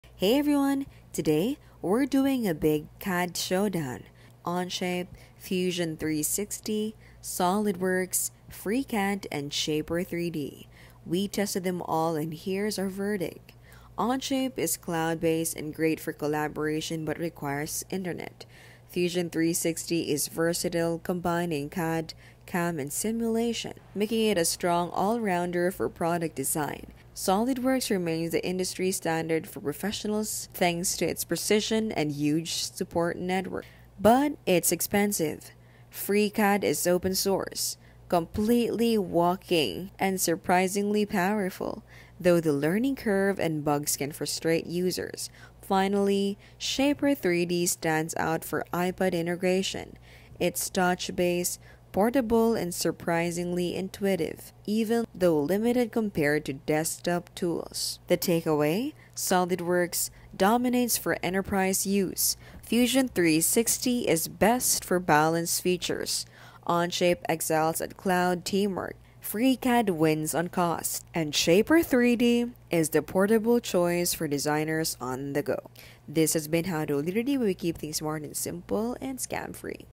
Onshape vs Fusion 360 vs Solidworks vs Freecad vs Shapr3d | (2025) The ULTIMATE CAD Software To Use
A fast, honest comparison of cloud, desktop, and tablet-first CAD 🧩⚙️ I test modeling speed, parametrics/constraints, assemblies/drawings, PDM/collab, CAM/CAE, sheet metal, import/export (STEP/IGES/DWG/DXF/STL), plugins, hardware needs, and pricing—so you can pick the right tool for product design, prototyping, and manufacturing in 2025 🏗️

When Onshape wins: browser-native parametric CAD, real-time co-editing, built-in PDM, release workflows, easy sharing

When Fusion 360 wins: integrated CAD/CAM/CAE, generative tools, good for prototyping to CNC/3D print in one app

When SOLIDWORKS wins: deep feature set for enterprise, mature drawings/assemblies, broad supplier ecosystem

When FreeCAD wins: open-source flexibility, no license cost, Python scripting for custom workflows

When Shapr3D wins: iPad/Mac direct+parametric modeling, Apple Pencil speed, quick concept → clean STEP/Parasolid export

Key checks: team collaboration, CAM pipeline, simulation needs, file interchange, add-on costs, offline vs cloud, TCO

Engineers, product designers, and makers moving from idea → prototype → production

Startups needing fast collaboration without heavy IT

Students and hobbyists balancing capability vs price

Pros who sketch on iPad and finish on desktop